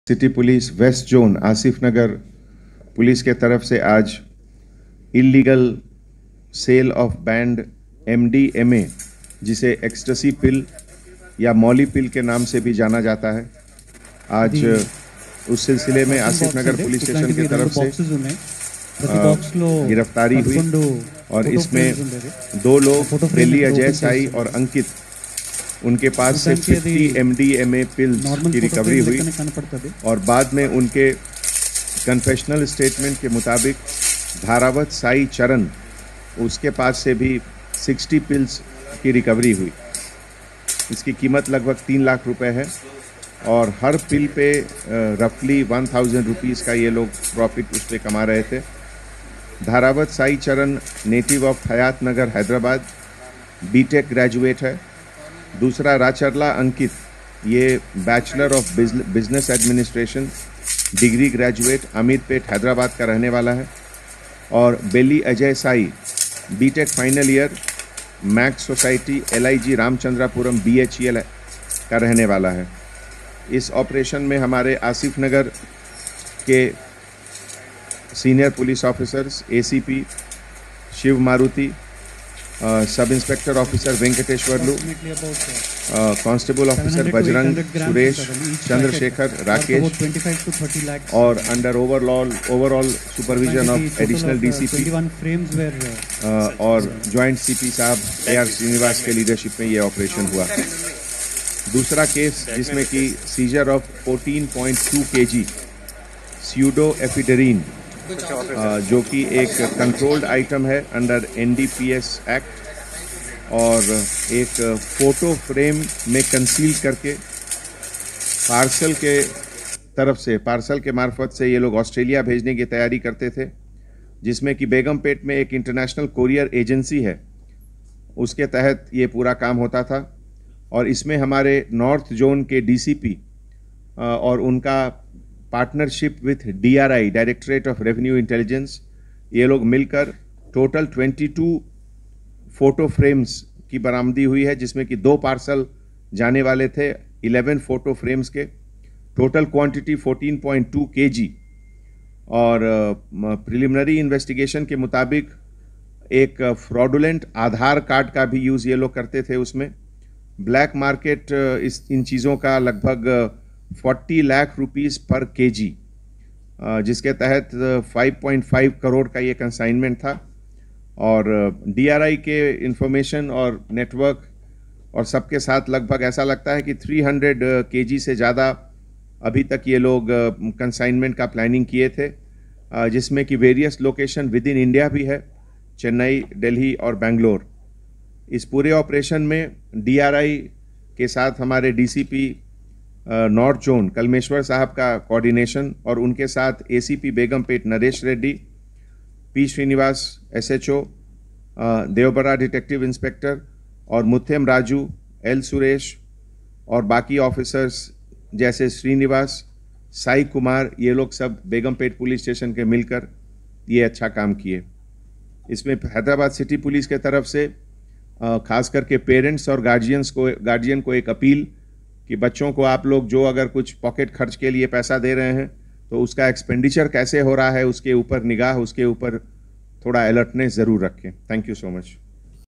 apprehend 2 person & seized psychotropic substanceMethylenedioxyMethamphetamine (MDMA) (50) Pills .
Today i.e on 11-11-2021, the Inspector of police, Asifnagar Police Station
received credible information that some persons are indulging in the sale of banned
psychotropic substance drugs at Mehdipatnam Bus Stop. Immediately Inspector
of Police deputed a team led by the Addl Inspector of Police to apprehend two
persons namely Belly Ajay Sai and Ankith and seized psychotropic substance
MethylenedioxyMethamphetamine (MDMA) (50) Pills and on their confessing
another 60 Pills were seized from Dharawath Sai Charan all worth Rs. 3 Lakhs.
The accused purchased the banned pills for a price of Rs. 1500/- each pill and sell
each pill to the consumer for a price of Rs. 2500/- and above.
M.O.